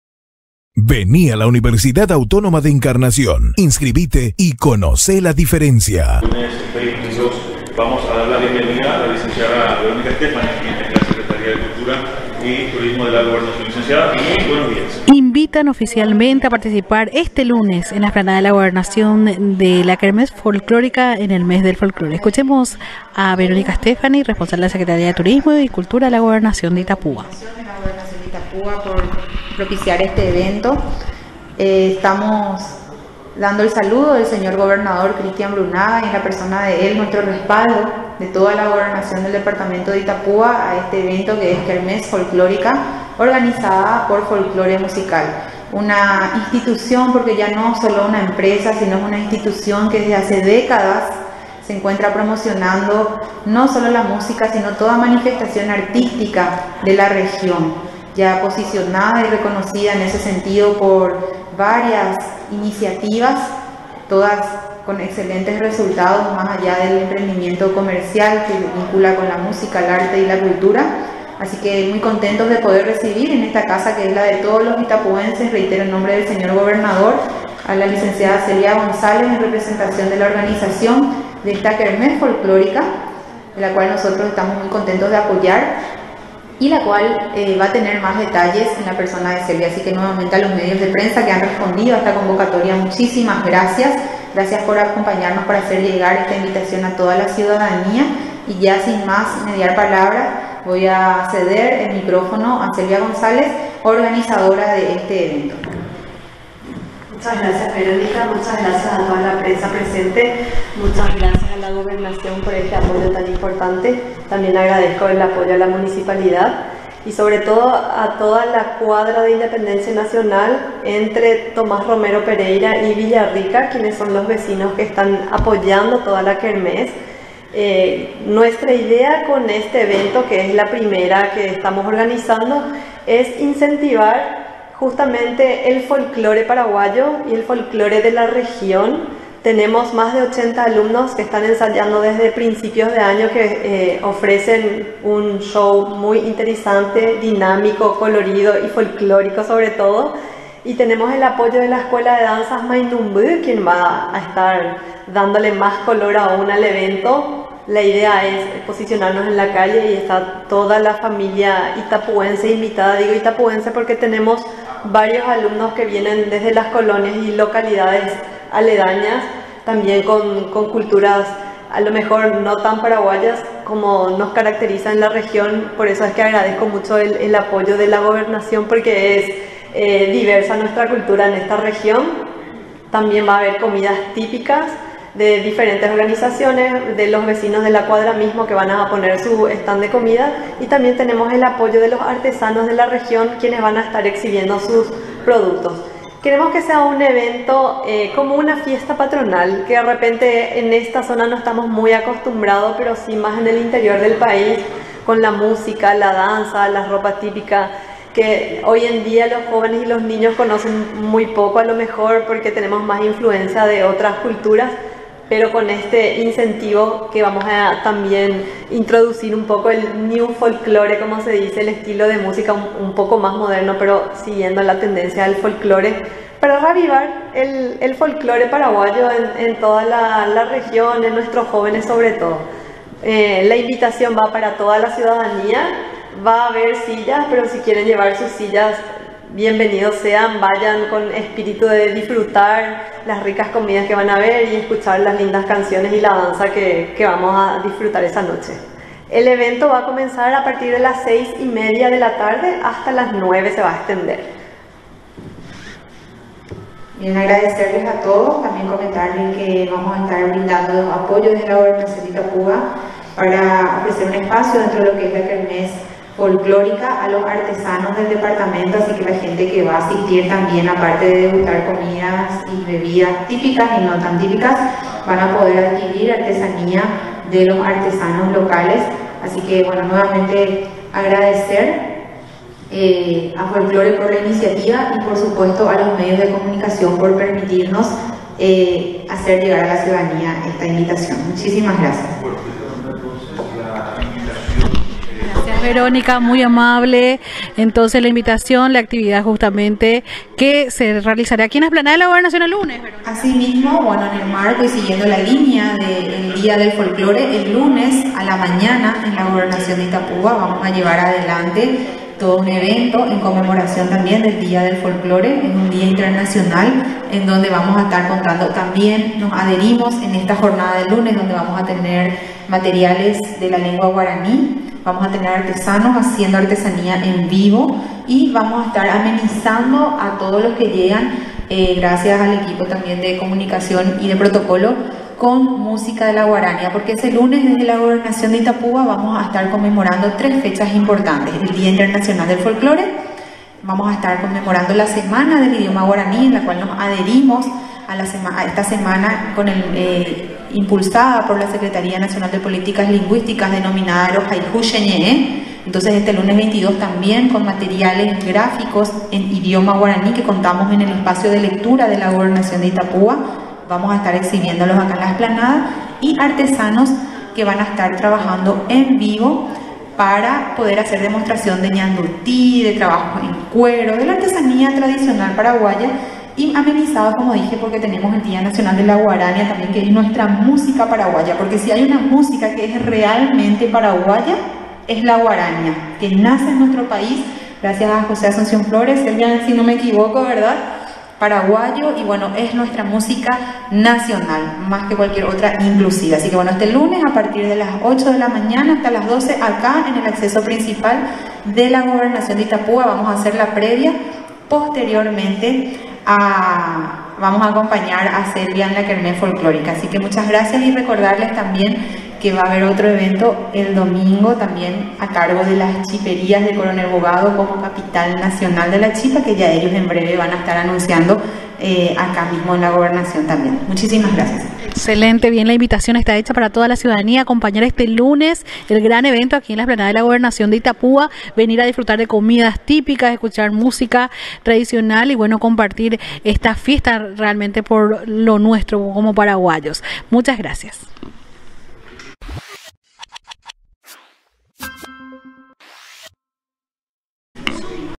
ESTE LUNES GRAN KERMESE FOLKLÓRICA EN ENCARNACIÓN
En la mañana de este viernes se anunciaron eventos que serán propiciados en el marco de la celebración del Día del Folklore, la semana de la Guarania y del Idioma Guaraní desde la Gobernación.

El lunes por la mañana habrá ambientación y actos que honren nuestra cultura en la Gobernación de Itapúa.

Luego, de 18:00 a 21:00hs. sobre la calle Independencia Nacional entre Villarrica y Tomás Romero Pereira de Encarnación, la Gobernación brindará apoyo a la  fiesta popular "Kermese Folklórica", organizada por Folklore Academia de Encarnación, dirigida por la Prof. Selvia González.

De esta fiesta tradicional participarán alumnos de la referida academia en la interpretación de un variado repertorio musical, más la actuación del Ballet Mainumby y reconocidos artistas locales como Juan Paredes, Emanuel Arapy, César Benítez y otros.

Asimismo, habrá juegos infantiles, se expondrán artesanías y se ofrecerá a la venta una interesante variedad de comidas típicas.

Verónica Stéfani, responsable de la Secretaría de Turismo y Cultura, destacó que el  Gobernador Christian Brunaga apoya esta y todas las manifestaciones artístico-culturales tendientes a defender, difundir y preservar el patrimonio folklórico regional y nacional.